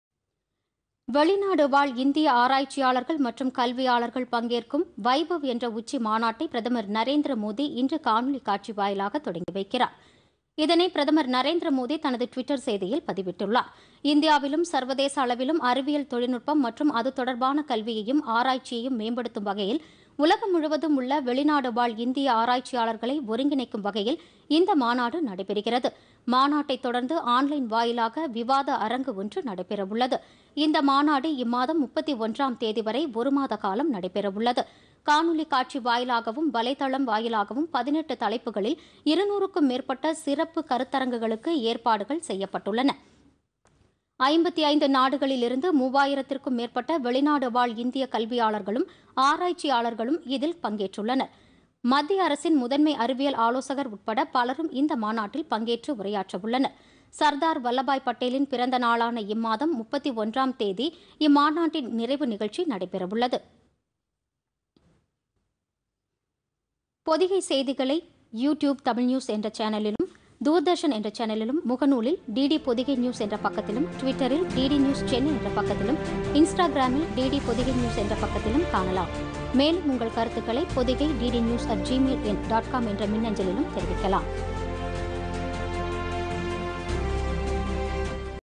வைபவ் உச்சி மாநாடு
வெளிநாடுவாழ் இந்திய ஆராய்ச்சியாளர்கள் மற்றும் கல்வியாளர்கள் பங்கேற்கும் வைபவ் என்ற உச்சி மாநாட்டை பிரதமர் நரேந்திர மோதி இன்று  தொடங்கி வைக்கிறார்